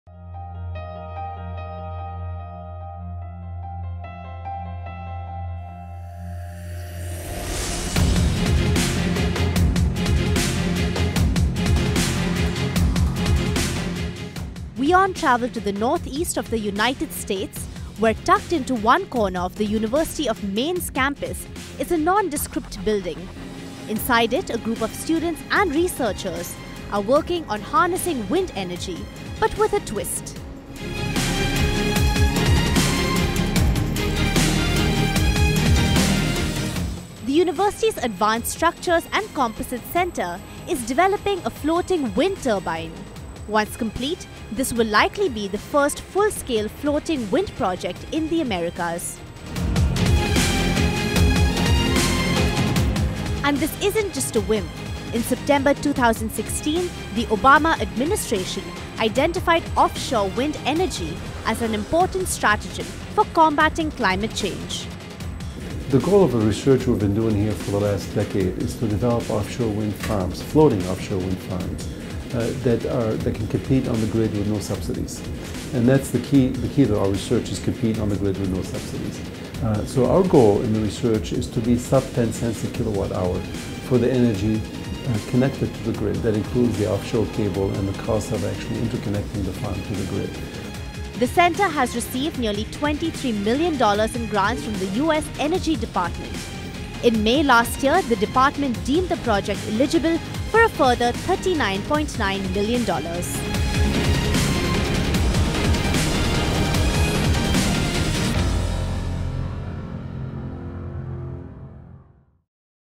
360 WION: University Of Maine, Offshore Wind Advanced Technology Demonstration
WION traveled to the northeast of the united states, where, tucked in to one corner of the University of Maine's campus is a nondescript building. Inside it a group of students and researchers are working on harnessing wind energy, but with a twist.